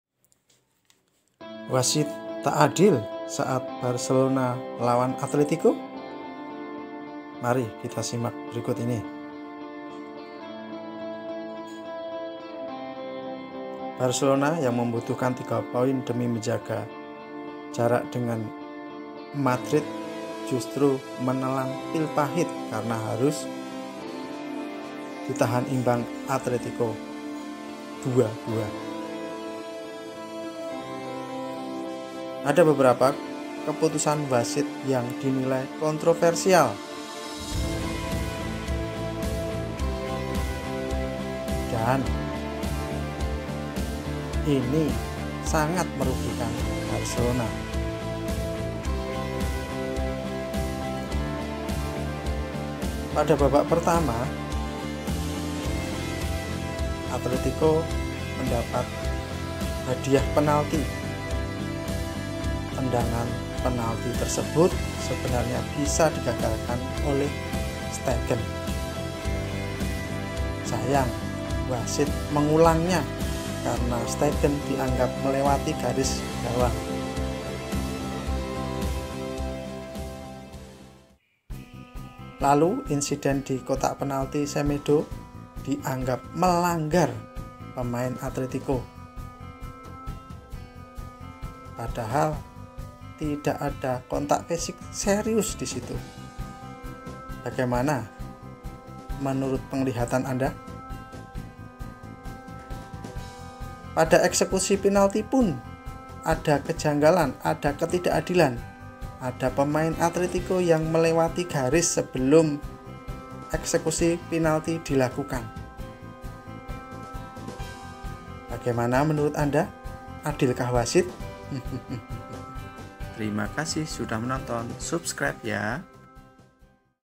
Barcelona 2-2 Atletico • Barcelona Dirugikan Wasit?
Forca Barca!
Saat harus menang demi persaingan merebut gelar dengan Madrid, Barcelona justru harus menelan pil pahit usai ditahan imbang Atletico.
Ada beberapa keputusan wasit yang dinilai kontroversial dan merugikan Barcelona.